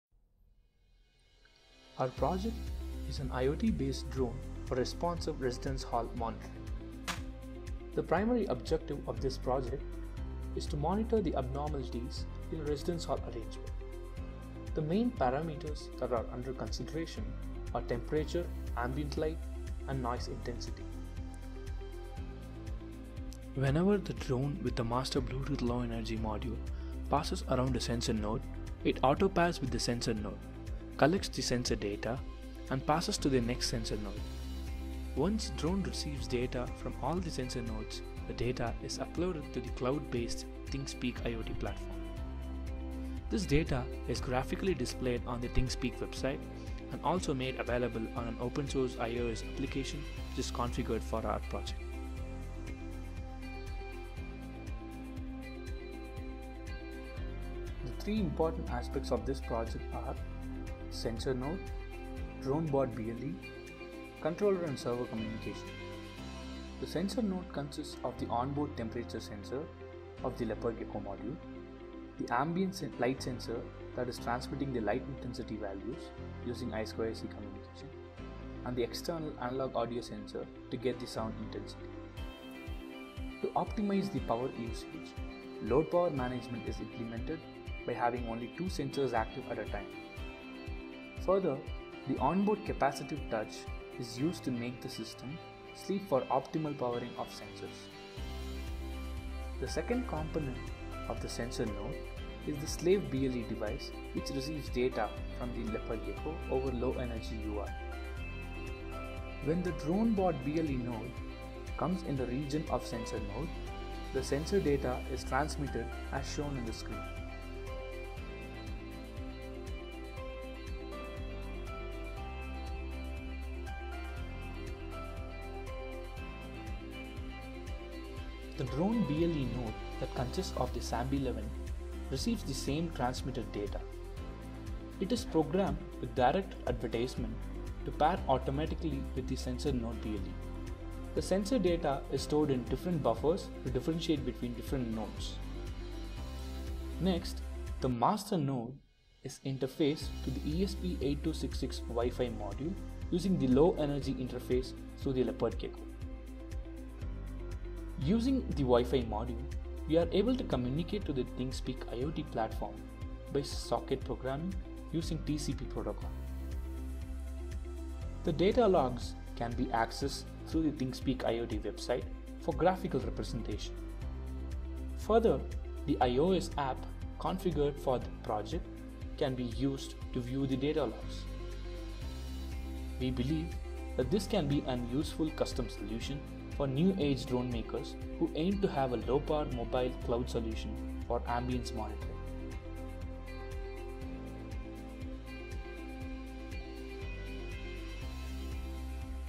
Drone Based Responsive Residence Monitoring System Using BLE, Thingspeak IoT Platform
An IoT based Drone/Bot system to determine and update the ambient conditions (Temperature, Sound Levels, Ambient Light etc.)  for responsive residence halls monitoring.

The project is aimed at monitoring local ambient abnormalities in a residence halls setup from a low power perspective.

• In this project we are using 2 sensor nodes comprising of a Leopard Gecko and the ATSAMB11 which sends data such as temperature, ambient light and sound intensity values.
• Within the sensor nodes during the first LETIMER period the node collects the temperature data and during the second period it collects the audio data. The ambient light value is calculated during both the periods. The sensor node sleeps during the third period to save power.
• Using LEUART we sending all these data to the ATSAMB11 to transmit it to the drone/bot BLE.
• When the Drone/Bot comes near a BLE devices and it receives a positive peer address it connects and pairs to the sensor node automatically using Directed Advertisement.
• The Drone/Bot BLE then stores this data in separate buffers.
• The Drone/Bot then disconnects and goes to the next sensor node to collect data.
• When all the sensor node’s data are stored in the buffer, we bring the Drone/Bot towards the controller.
• The controller can then enable to send the data to the server using ESP-8266 module. This is achieved by interfacing SAMB11 to Leopard Gecko through LEUART and it uses the same LEUART to send data to the ESP-8266 module using AT commands.